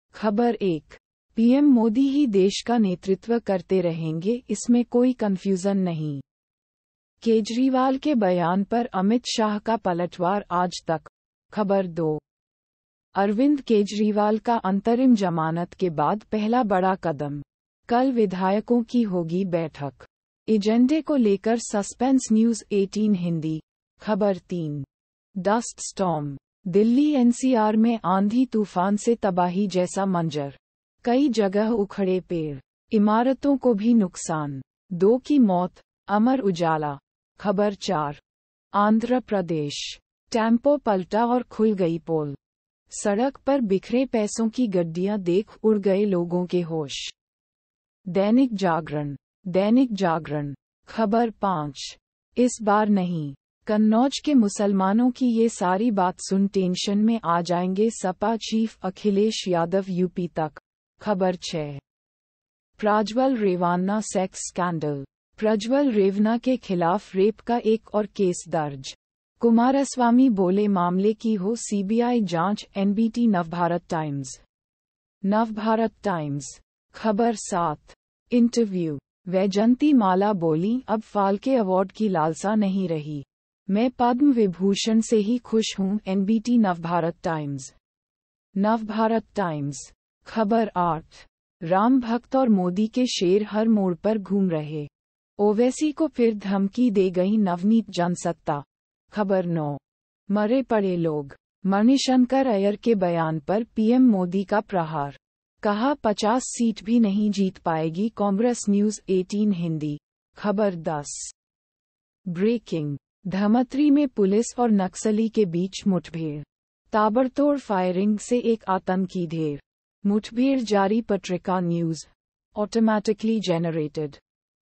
Hindi News (भारत)- 11/05/2024,07:07 PM GMT+5:30
ख़बर 1:'PM मोदी ही देश का नेतृत्व करते रहेंगे इसमें कोई कन्फ्यूजन नहीं...', केजरीवाल के बयान पर अमित शाह का पलटवार - Aaj Tak
ख़बर 2:अरविंद केजरीवाल का अंतरिम जमानत के बाद पहला बड़ा कदम, कल विधायकों की होगी बैठक, एजेंडे को लेकर सस्पेंस - News18 हिंदी
ख़बर 3:Dust Storm: दिल्ली-NCR में आंधी-तूफान से तबाही जैसा मंजर, कई जगह उखड़े पेड़, इमारतों को भी नुकसान; दो की मौत - अमर उजाला
ख़बर 4:Andhra Pradesh: टैम्पो पलटा और खुल गई पोल... सड़क पर बिखरे पैसों की गड्डियां देख उड़ गए लोगों के होश.. - दैनिक जागरण (Dainik Jagran)
ख़बर 5:इस बार नहीं…कन्नौज के मुसलमानों की ये सारी बात सुन टेंशन में आ जाएंगे सपा चीफ अखिलेश यादव - UPTak
ख़बर 6:Prajwal Revanna Sex Scandal: प्रज्वल रेवन्ना के खिलाफ रेप का एक और केस दर्ज, कुमारस्वामी बोले- मामले की हो सीबीआई जांच - NBT नवभारत टाइम्स (Navbharat Times)
ख़बर 7:Interview: वैजयंती माला बोलीं- अब फाल्के अवॉर्ड की लालसा नहीं रही, मैं पद्म विभूषण से ही खुश हूं - NBT नवभारत टाइम्स (Navbharat Times)
ख़बर 8:राम भक्त और मोदी के शेर हर मोड़ पर घूम रहे... , ओवैसी को फिर धमकी दे गईं नवनीत - Jansatta
ख़बर 9:'मरे पड़े लोग...' मणिशंकर अय्यर के बयान पर पीएम मोदी का प्रहार, कहा- 50 सीट भी नहीं जीत पाएगी कांग्रेस - News18 हिंदी
ख़बर 10:Breaking: धमतरी में पुलिस और नक्सली के बीच मुठभेड़, ताबड़तोड़ फायरिंग से एक आतंकी ढेर, मुठभेड़ जारी - Patrika News

Automatically Generated...